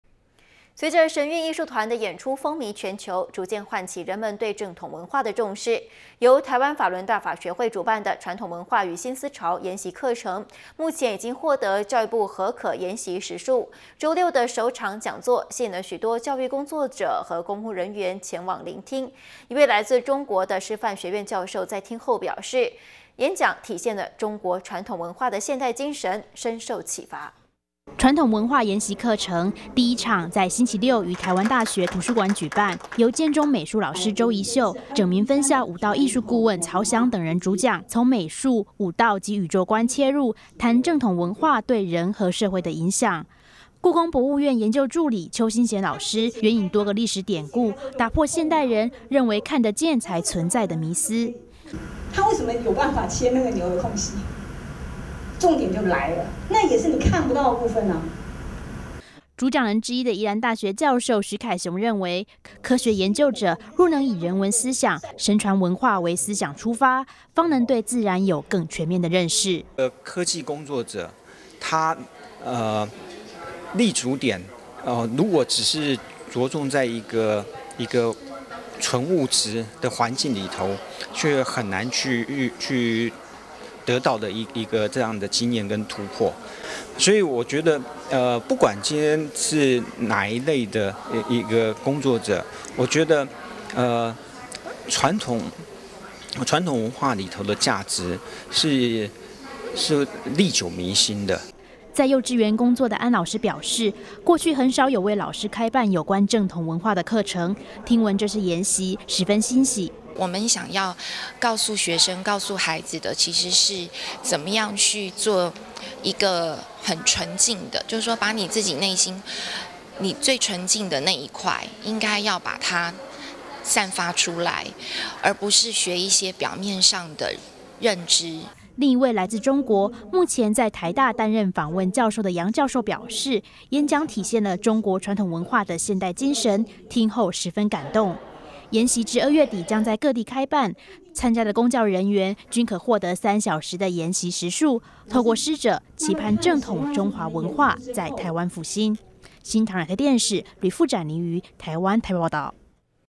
【教育學習】傳統文化研習課程 首場台大開講
【教育學習】傳統文化研習課程 首場台大開講(NTDAPTV) 隨著神韻藝術團的演出風靡全球，逐漸喚起人們對正統文化的重視。由台灣法輪大法學會主辦的傳統文化與新思潮研習課程，目前已獲教育部核可研習時數。週六的首場講座，吸引許多教育工作者和公務人員前往聆聽。一位來自中國的師範學院教授在聽後表示，演講體現了中國傳統文化的現代精神，深受啟發。【新唐人亞太台2013年1月19日訊】

傳統文化研習課程，第一場在星期六於台灣大學圖書館舉辦，由建中美術老師周怡秀、拯民分校舞蹈藝術顧問曹翔等人主講，從美術、舞蹈及宇宙觀切入，談正統文化的對人和社會的影響。

故宮博物院研究助理邱馨賢老師，援引多個歷史典故，打破現代人「認為看的見才存在」迷思。

故宮博物院書畫處研究助理 邱馨賢：「他為什麼有辦法切牛的空隙，重點就來了，那也是你看不到的部分。」

主講人之一的宜蘭大學教授許凱雄認為，科學研究者，若能以人文思想、神傳文化為思想出發，方能對自然有更全面的認識。

宜蘭大學教授 許凱雄：「科技工作者，他的立足點如果只是著重在純物質的環境裏頭，很難得到這樣一個經驗和突破。我覺得不管是哪一類的工作者，傳統文化裡頭的價值是歷久彌新的。」

在幼稚園工作的安老師表示，過去很少為老師開辦有關正統文化的課程，聽聞這次研習，十分欣喜。

幼兒園老師 安鳳琪：「我們想要告訴學生，告訴孩子的是，怎麼樣去做一個很純淨的，把你自己內心，你最純淨的的那一塊，應該要把它散發出來，而不是學一些表面上的認知。」

另一位來自中國，目前在台大擔任訪問教授的楊教授表示，演講體現了中國傳統文化的現代精神，聽後十分感動。

研習至二月底將在各地開辦，參加的公教人員，均可獲得三小時的研習時數。透過師者，期盼正統中華文化，在台灣復興。